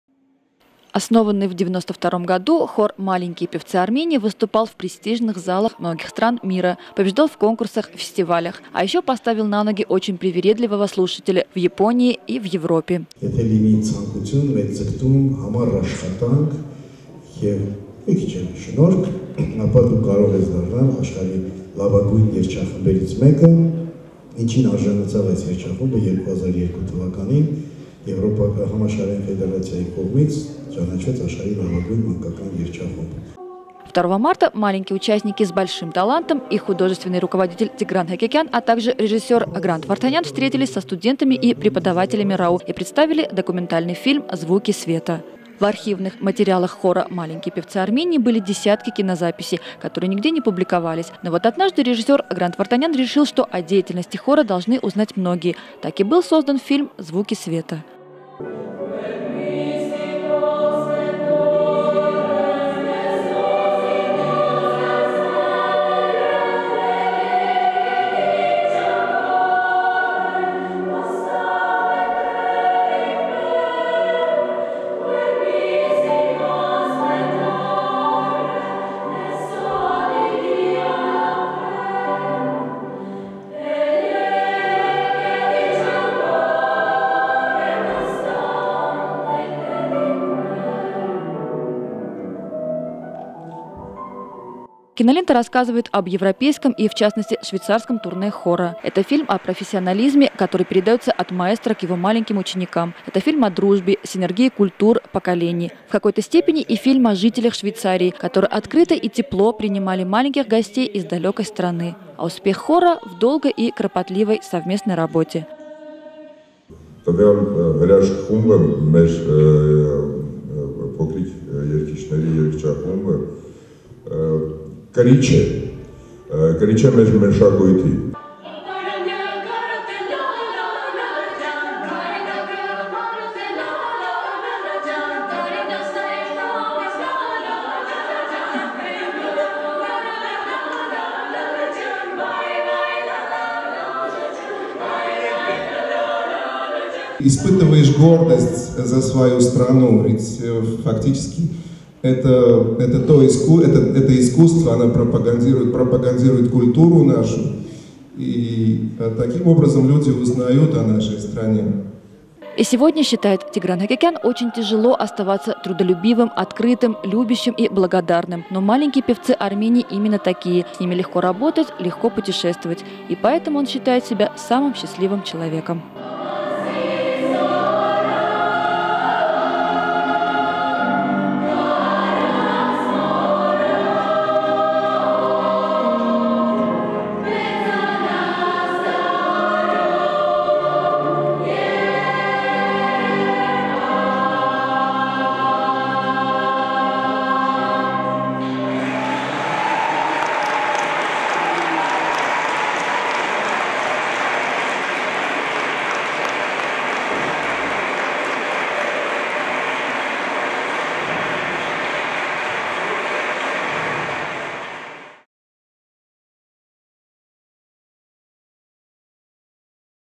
Хор "Маленькие певцы Армении" в РАУ
2-го марта в Доме культуры Российско-Армянского университета прошла встреча с хором “Маленькие певцы Армении”, его художественным руководителем и главным дирижером Тиграном Экекяном, а также режиссером Грантом Варданяном.
В рамках мероприятия состоялся показ документального фильма “Звуки света”, посвященного творческой деятельности хора.